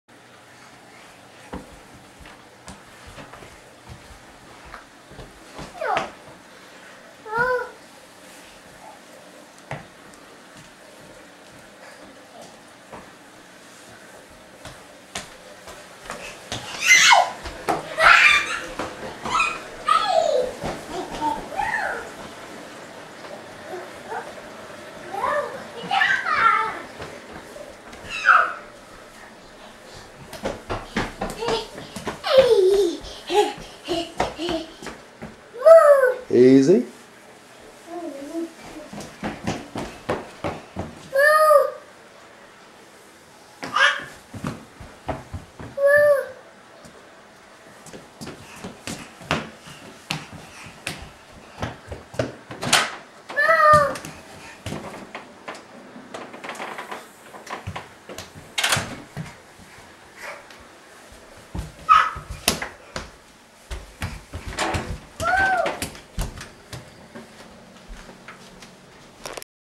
Lead Her Astray, My Son.
Nate does his best to lead Baby Avery outside the house.